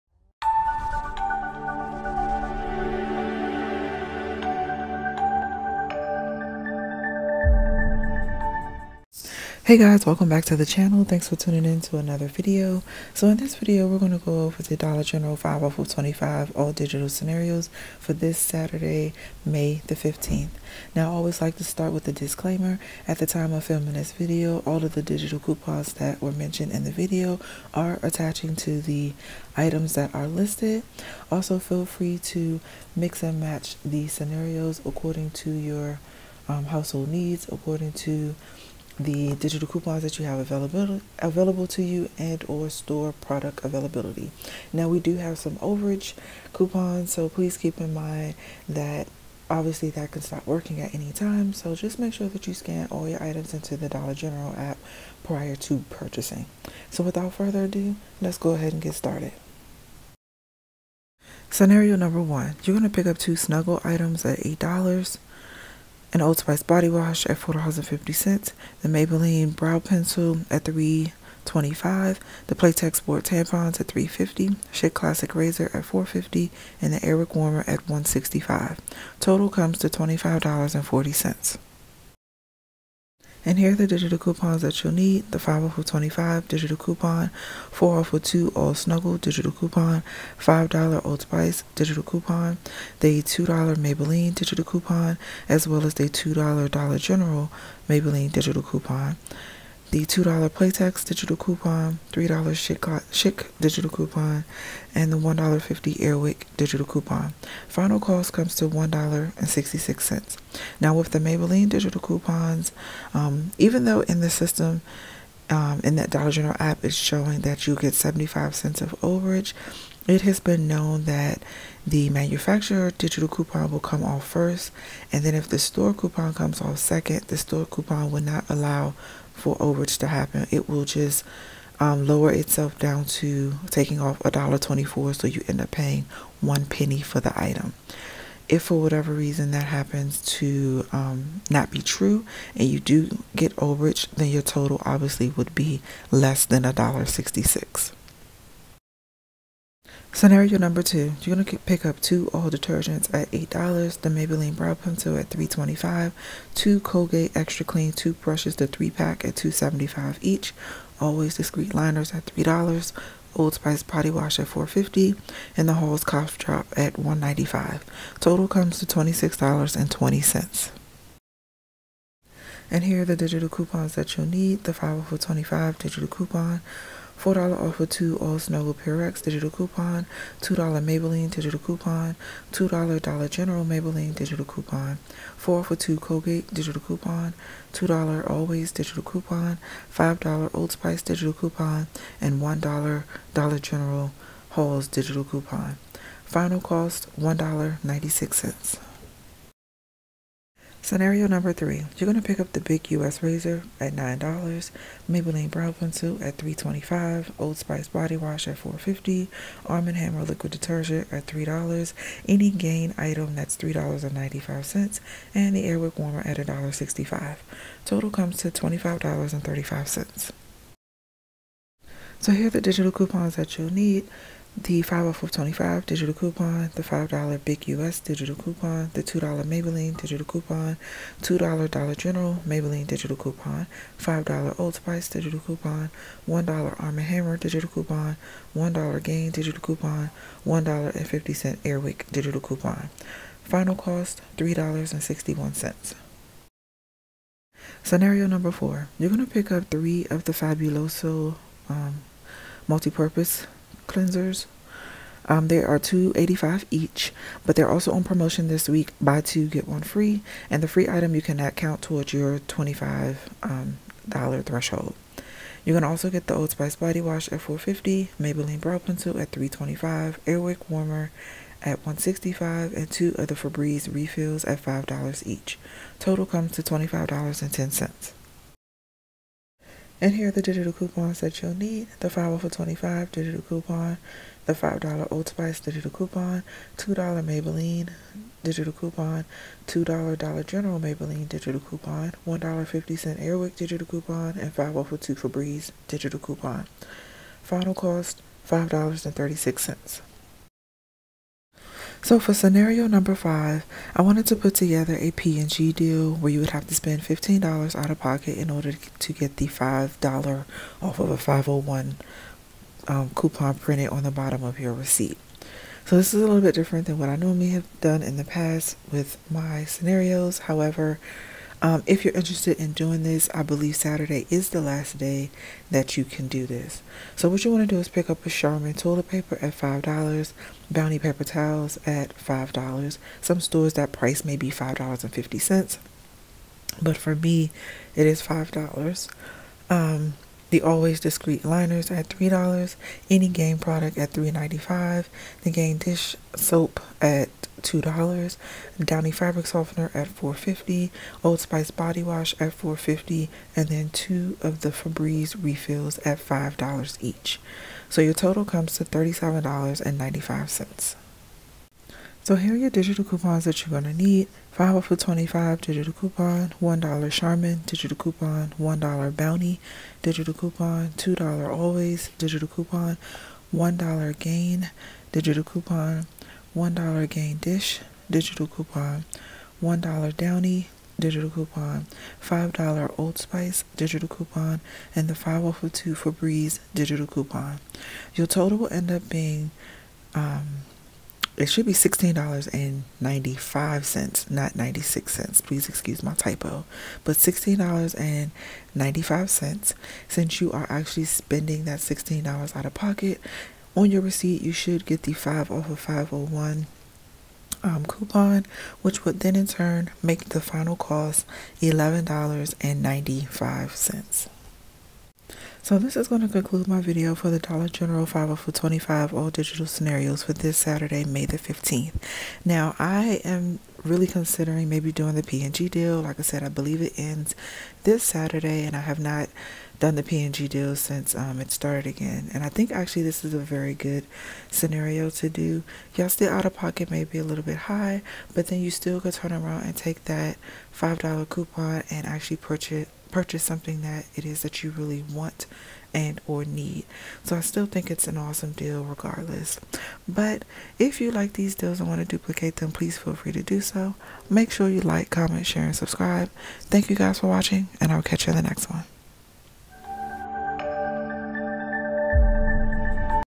Dollar General $5/$25 All Digital Scenarios for Saturday, May 15th | LadyMekah
Hey guys. welcome back to the channel. Here is my Dollar General $5/$25 All Digital Scenarios for Saturday, May 15th. I hope you enjoy the video.

Shopkick - BEST637909